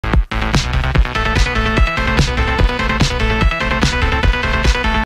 Éloge géométrique - Carré
Animation en motion design du carré.
Réalisation sous Adobe After Effects.